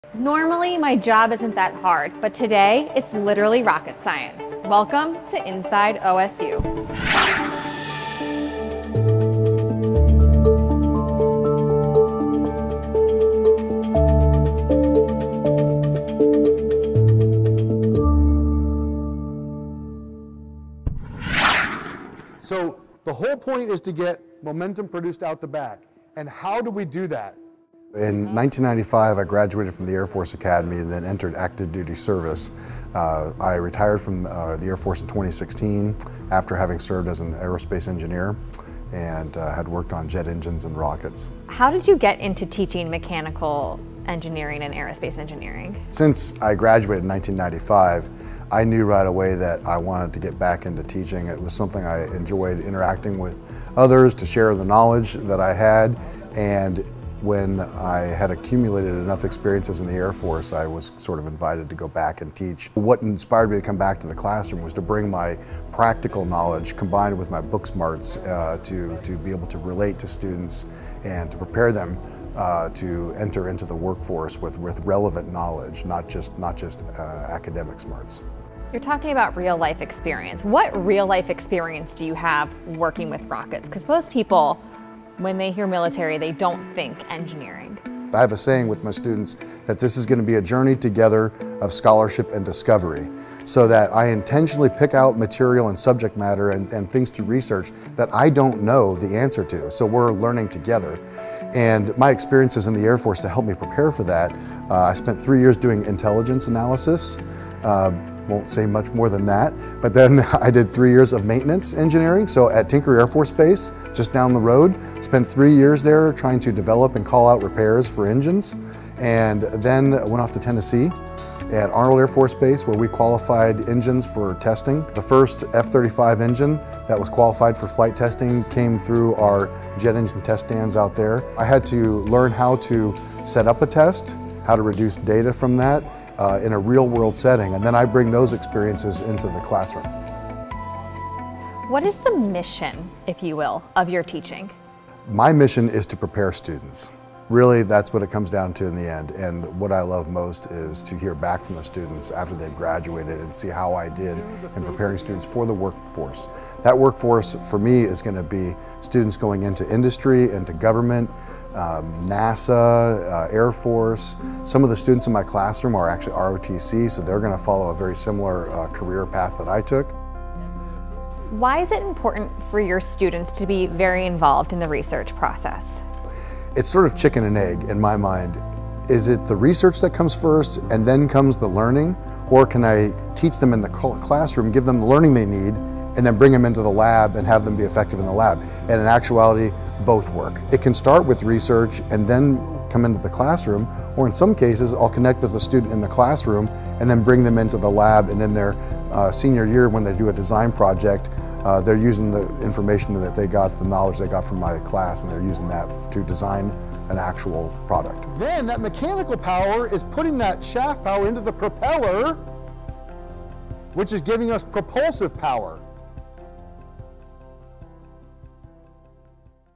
Launching Careers in Aerospace
Dr. Kurt Rouser graduated from the Air Force Academy in 1995 and immediately entered active duty service as an Aerospace Engineer – where he worked on rockets and jet engines. After over 20 years of service, he retired from the Air Force in 2016. His love of interacting with others and desire to share his real world experience led him to becoming a professor in Oklahoma State’s Mechanical and Aerospace Engineering program. Dr. Rouser specializes in aerospace propulsion and power, but his passion is in preparing students to enter the workforce with relevant knowledge beyond their academic needs. Meghan Robinson has more…